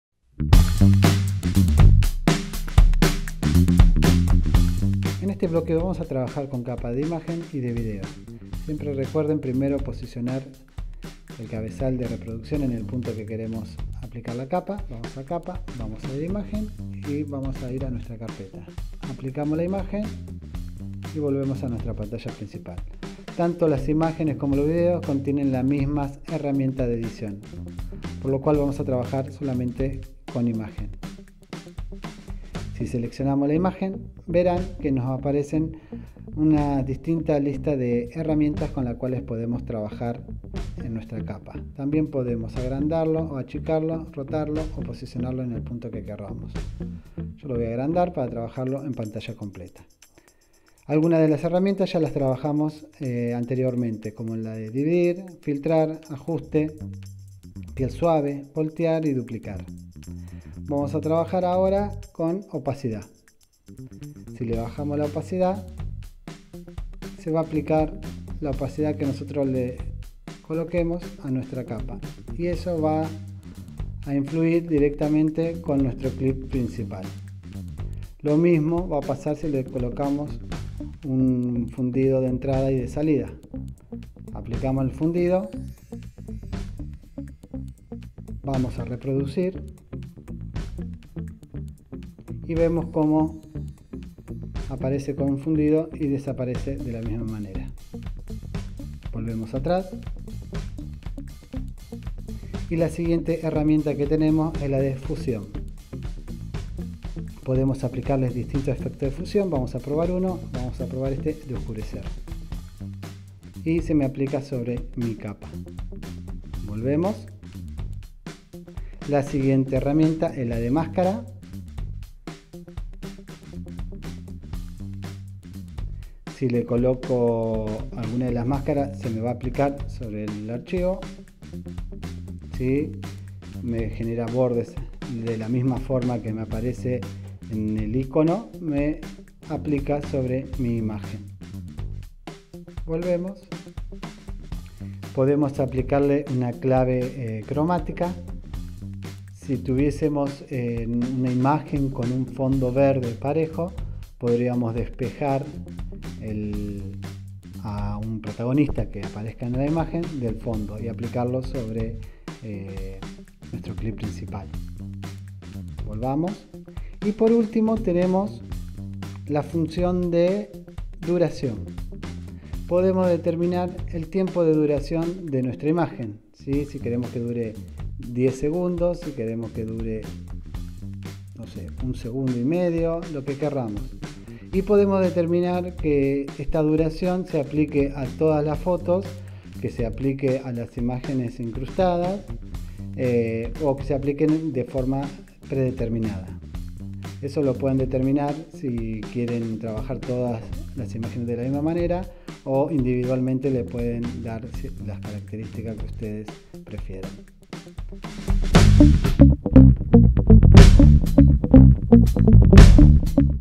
PowerDirector 04_Capas de Imagen y Video
PowerDirector es una herramienta de edición de vídeo muy completa, gracias a la que podremos crear nuestras propias películas utilizando todo tipo de material: fotografías, clips de vídeo y audio.